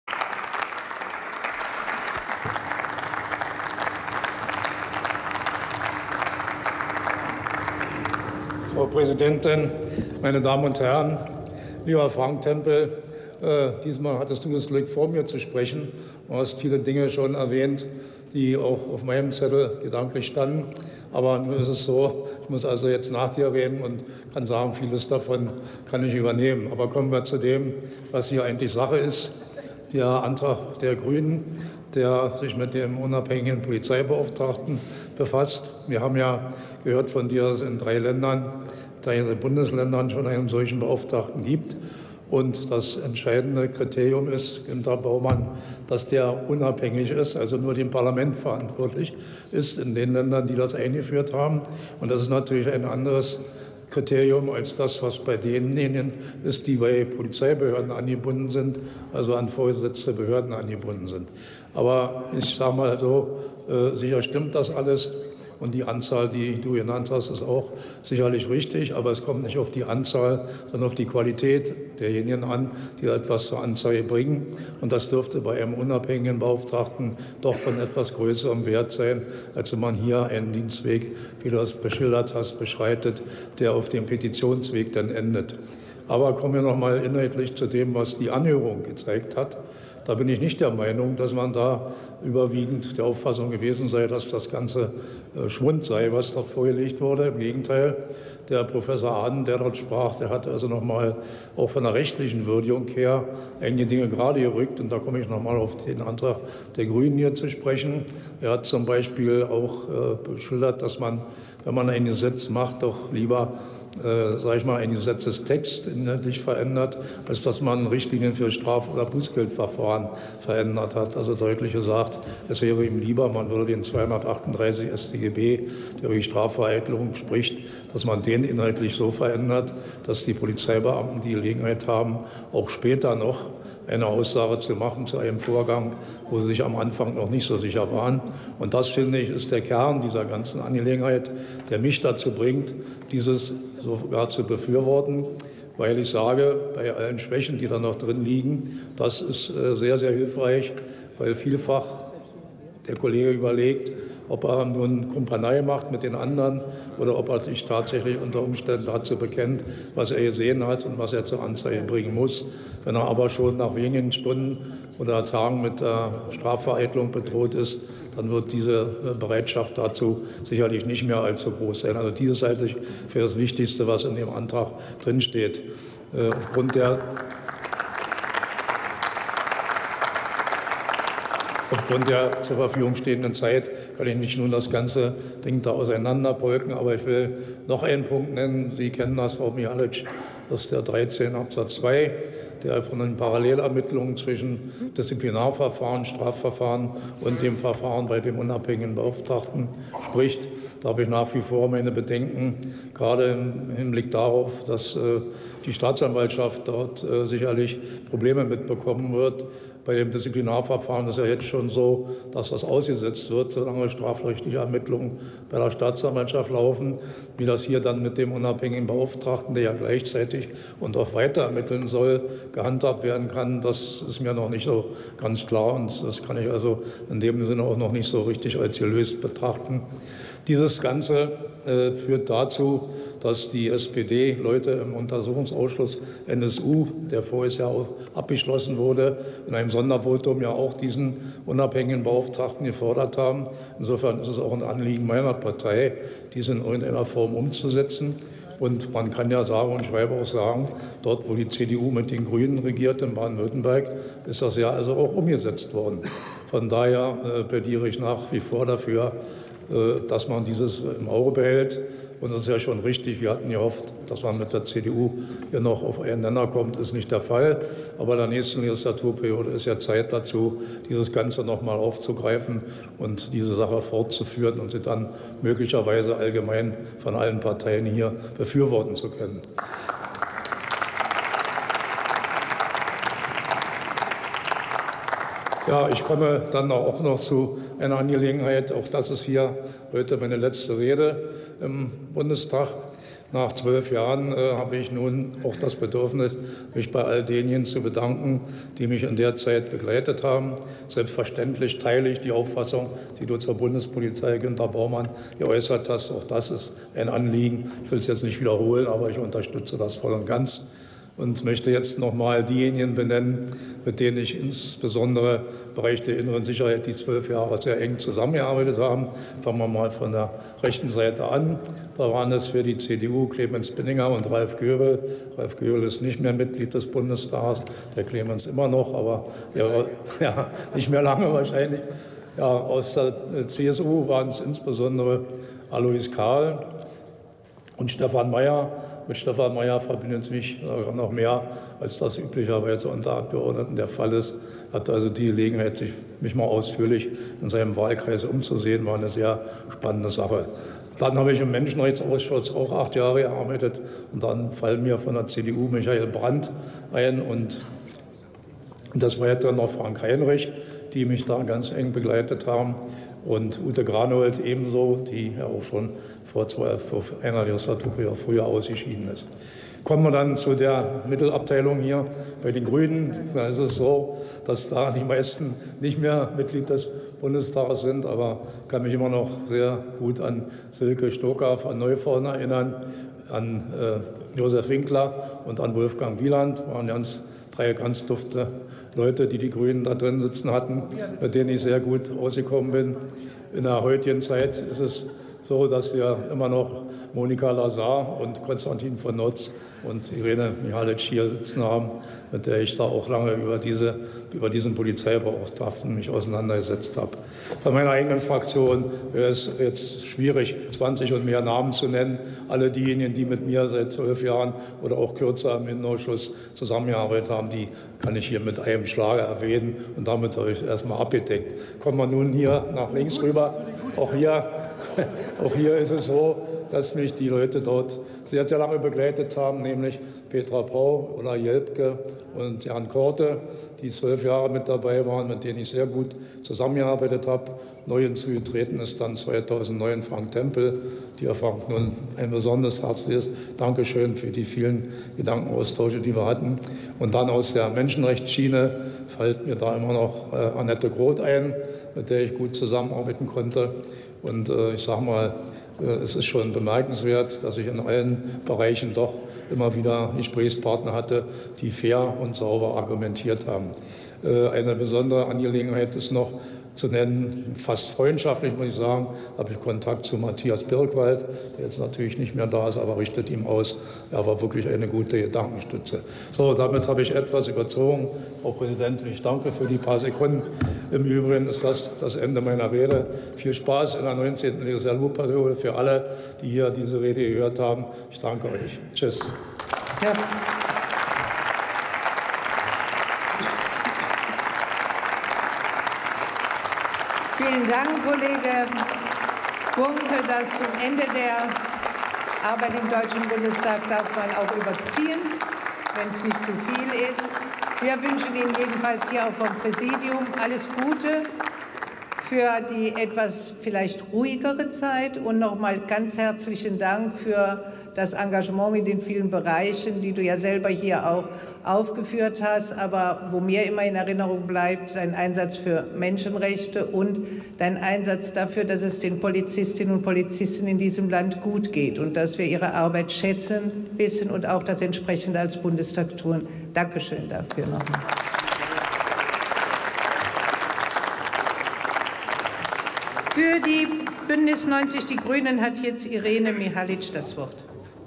Wolfgang Gunkel: Bundespolizeibeauftragtengesetz [Bundestag 29.06.2017]
a) Zweite und dritte Beratung des von der Fraktion BÜNDNIS 90/DIE GRÜNEN
eingebrachten Entwurfs eines Gesetzes über die unabhängige Polizeibeauftragte oder den unabhängigen Polizeibeauftragten des Bundes (Bundespolizeibeauftragtengesetz – BPolBeauftrG)
Drucksache 18/7616
Beschlussempfehlung und Bericht des Innenausschusses (4. Ausschuss)
Drucksache 18/12826
b) Beratung der Beschlussempfehlung und des Berichts des Innenausschusses (4. Ausschuss)
Aufklärung polizeilichen Fehlverhaltens erleichtern – Ergänzung zum Entwurf eines Gesetzes über die unabhängige Polizeibeauftragte oder den unabhängigen Polizeibeauftragten des Bundes (Bundespolizeibeauftragtengesetz – BPolBeauftrG)
c) Beratung der Beschlussempfehlung und des Berichts des Ausschusses für Wahlprüfung, Immunität und Geschäftsordnung (1. Ausschuss)
Änderung der Geschäftsordnung des Deutschen Bundestages
hier: Umsetzung des Gesetzes über die unabhängige Polizeibeauftragte oder den unabhängigen Polizeibeauftragten des Bundes (Bundespolizeibeauftragtengesetz – BPolBeauftrG)

18. Deutscher Bundestag
243. Plenarsitzung
Tagesordnungspunkt: Bundespolizeibeauftragtengesetz
Redner: Gunkel, Wolfgang, SPD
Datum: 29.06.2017
Plenarprotokoll:
UVID:7125680